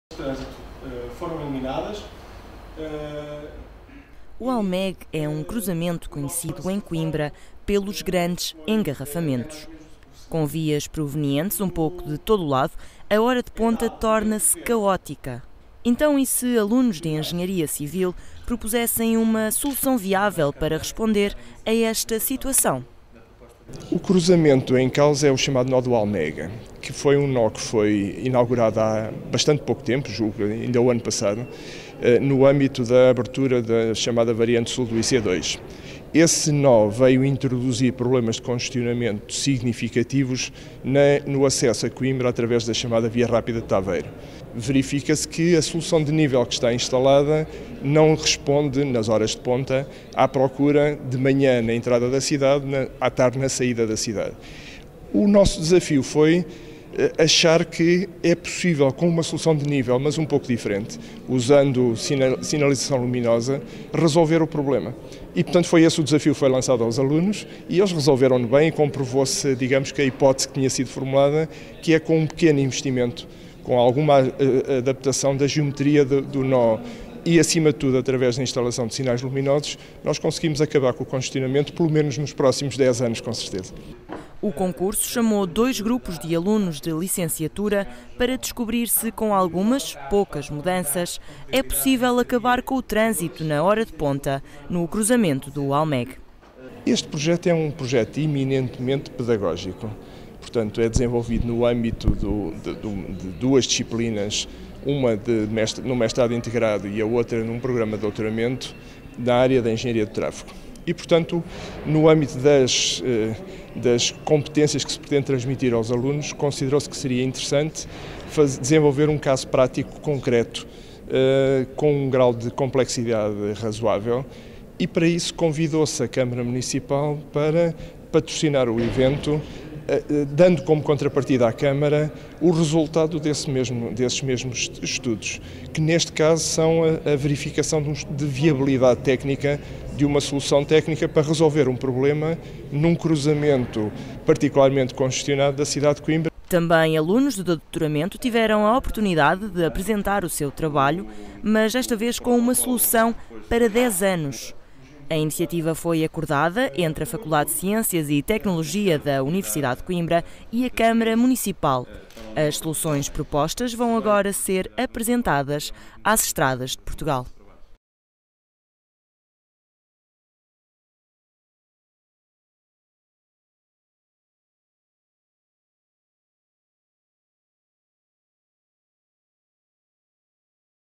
Alunos propõem solução para o Almegue
E se alunos de Engenharia Civil propusessem uma solução viável para responder aos engarrafamentos do cruzamento do Almegue?